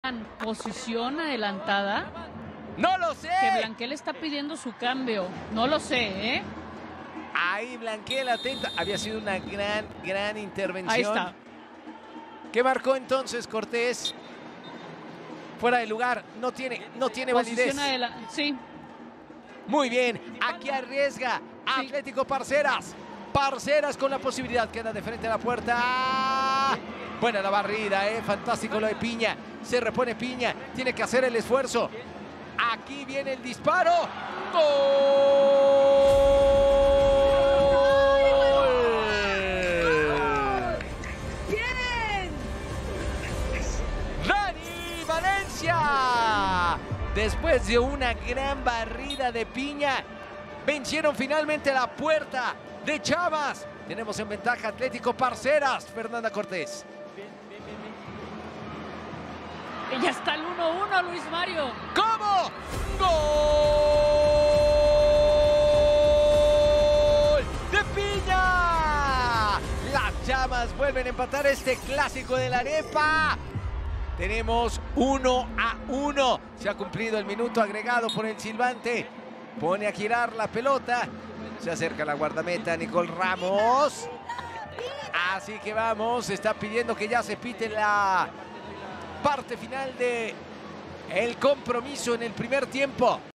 ¡EMPATE DE LAS CHAMAS a los segundos del 1-0! | Queens League
🥵 ¡¿QUÉSTAPASANDAAAA?! No dejaron ni celebrar a Atlético Parceras y Las Chamas pusieron el 1-1 🔥⚽